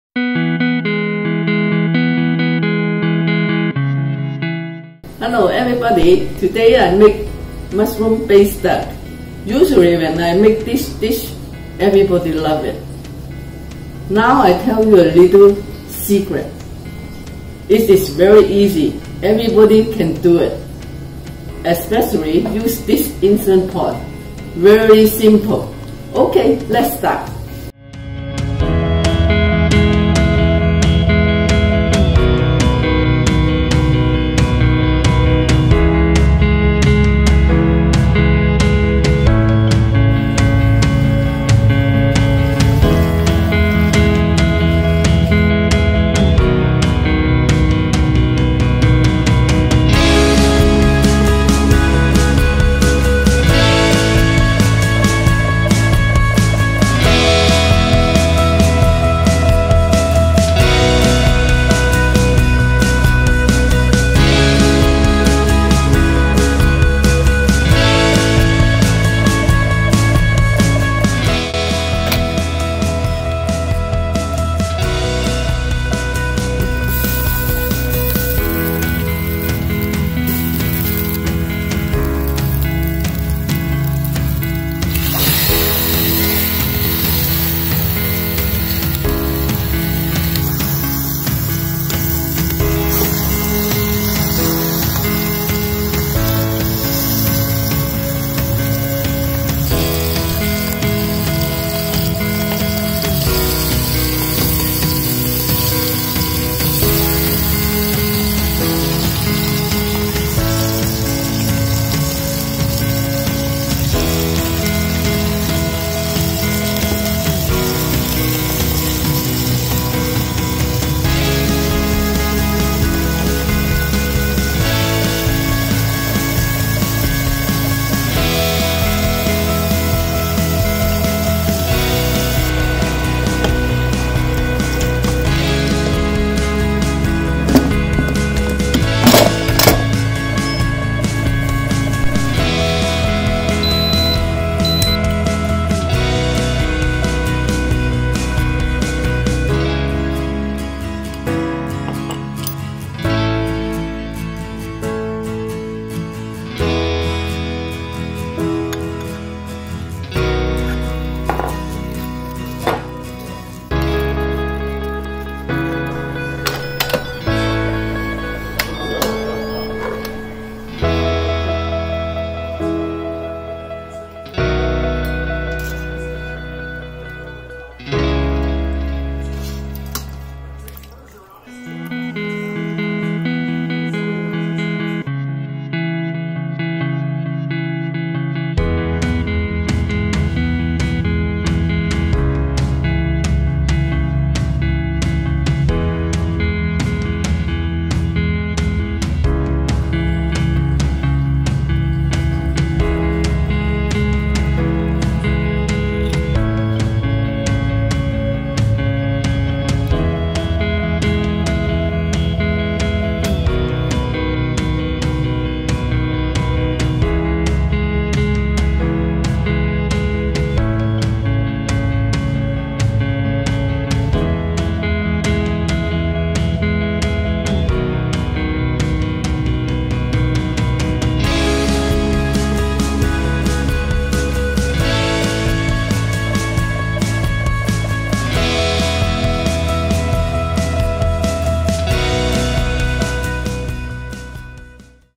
Impressive Braised Duck dish made super easy with Instant Pot | Home Cooking with Mom
Duck is one of those dishes everyone thinks is tricky. This is another one of those crowd pleasers that my friends and family think is really hard to make. With the Instant Pot, it's super easy and quick! I've been asked for this recipe so many times and now I'm sharing it with the world.

I'm using a 6 quart Instant Pot.

Ingredients
- Duck
- Chinese Mushrooms
- Napa Cabbage

Seasoning
- Oyster Sauce
- Dark Soy sauce
- Chinese Mushroom
- Rock Sugar
- Ginger
- Garlic
-  Star Anise
- Green Onion
- Salt
- Parsley

❤ Monica